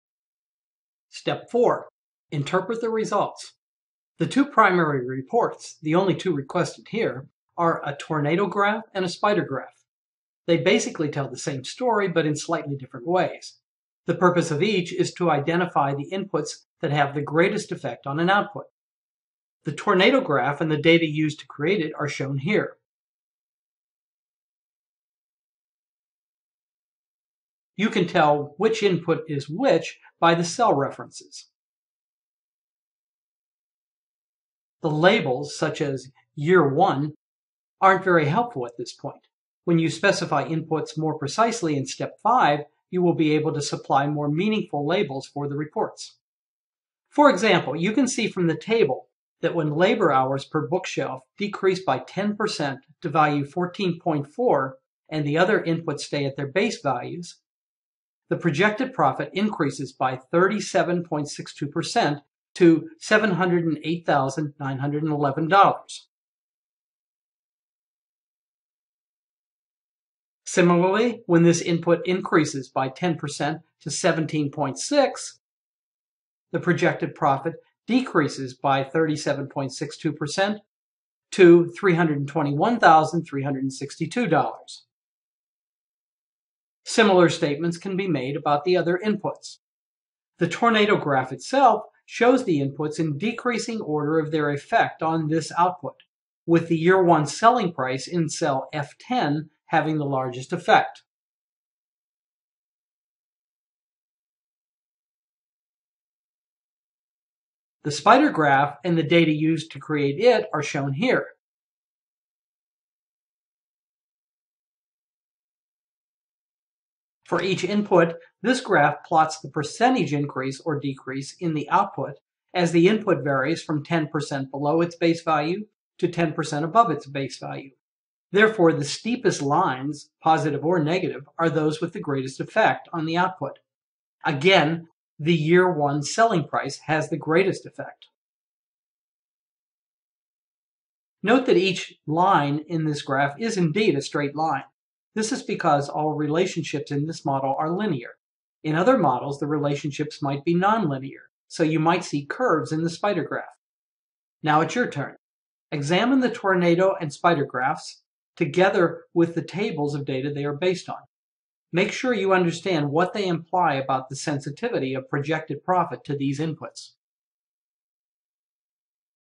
TopRank Quick Start - Step 4: Interpret the Results
These short, interactive tutorials are designed to teach you how to use TopRank software.
This is the video that corresponds Step 4.